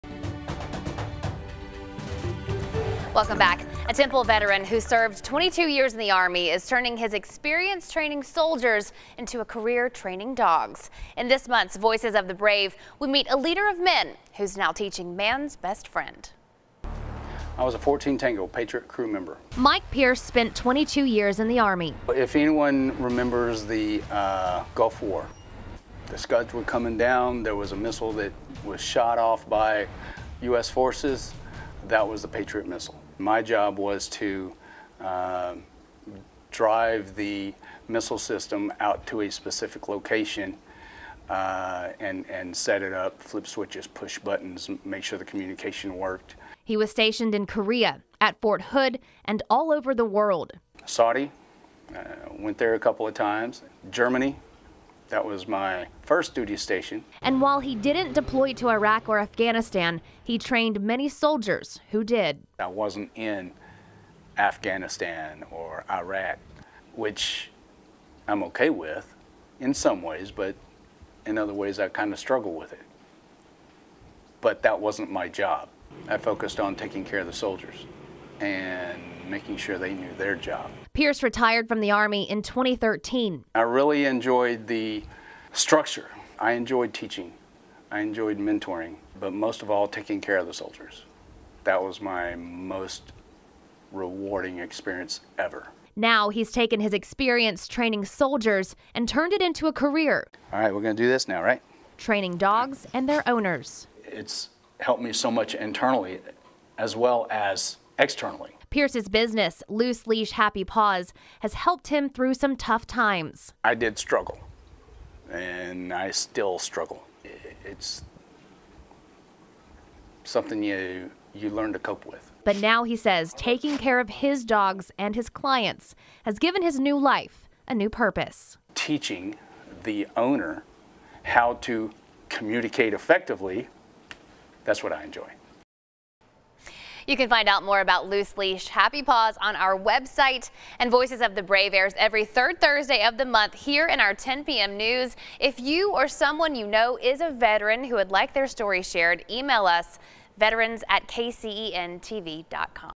Voices of the Brave: Army veteran goes from leading men to training dogs
In this month's Voices of the Brave, evening anchor Leslie Draffin spoke to a Gulf War veteran. Mike Pierce transitioned from being a leader of men, to teaching man's best friend.